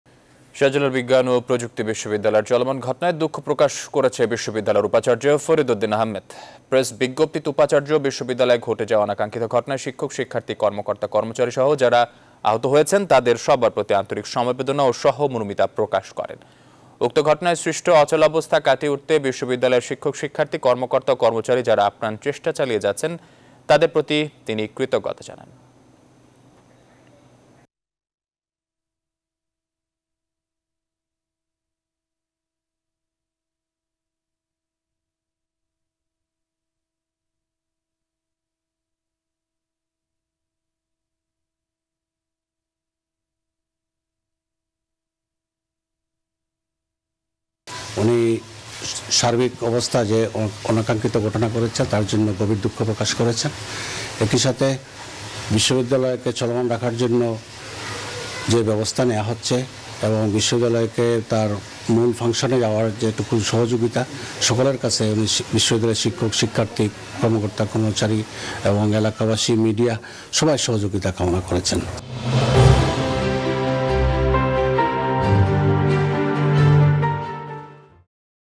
১৬ জানুয়ারির ঘটনায় শিক্ষার্থীদের কাছে দুঃখ প্রকাশ করেছেন শাবিপ্রবির উপাচার্য 12Feb.22| SUST
শাহজালাল বিজ্ঞান ও প্রযুক্তি বিশ্ববিদ্যালয়ে চলমান ঘটনায় দুঃখ প্রকাশ করেছেন বিশ্ববিদ্যালয়ের উপাচার্য ফরিদ উদ্দিন আহমেদ।

প্রেস বিজ্ঞপ্তিতে উপাচার্য, বিশ্ববিদ্যালয়ে ঘটে যাওয়া অনাকাঙ্ক্ষিত ঘটনায় শিক্ষক, শিক্ষার্থী, কর্মকর্তা-কর্মচারীসহ যারা আহত হয়েছেন, তাদের সবার প্রতি আন্তরিক সমবেদনা ও সহমর্মিতা প্রকাশ করেন। উক্ত ঘটনায় সৃষ্ট অচলাবস্থা কাটিয়ে উঠতে বিশ্ববিদ্যালয়ের শিক্ষক, শিক্ষার্থী , কর্মকর্তা ও কর্মচারী যারা আপ্রাণ চেষ্টা চালিয়ে যাচ্ছেন তাদের প্রতি তিনি কৃতজ্ঞতা জানান।

Our other YouTube channels :
News24 Official Address:
NEWS24
371/A
Bashundhara Road
Block – D
Bashundhara Residential Area
Dhaka - 1229